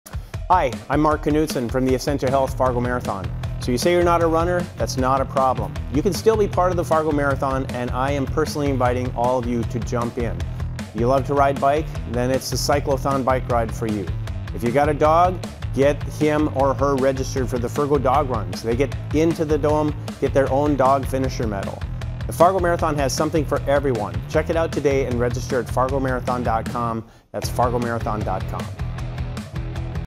Fargo Marathon 2023
Join us as we commemorate our 19th year of races full of tradition, heart, and good ol’ fashion Fargo fun! The Fargo Marathon features full and half marathons, a 10k, 5k, Youth Run, Dog Run and Cyclothon, as well as the GoFar Challenge. Come check out the week-long celebration of events, with over 50 live bands, a town-wide cheering section, and the 27th Mile Post-Race Party rockin' with music, food, cold beer and fun!